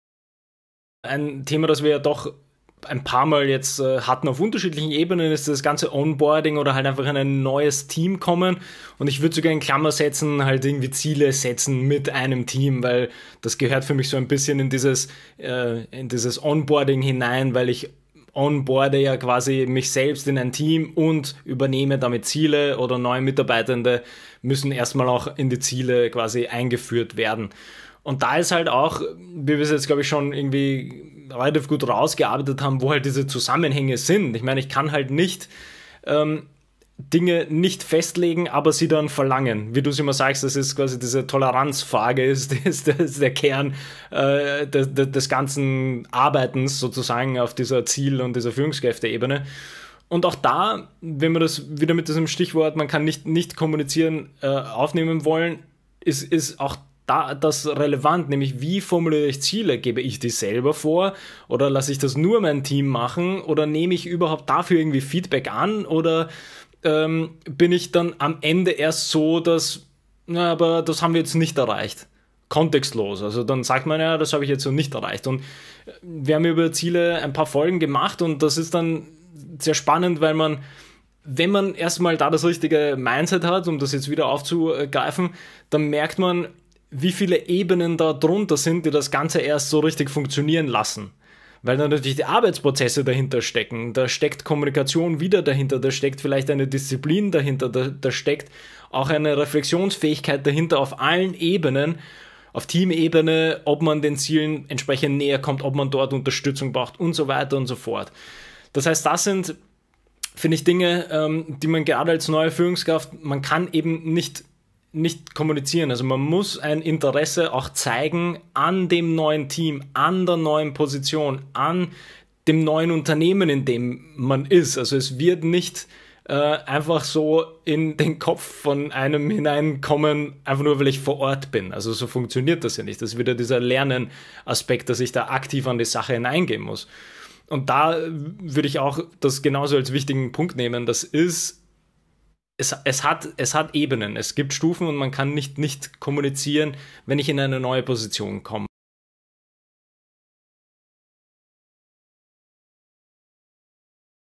Worauf muss eine junge Führungskraft besonders achten?
In diesem kurzen Clip spricht Bence über den Weg der Kommunikation (rund um das Team und Zielen) als neue junge Führungskraft aktiv gehen muss.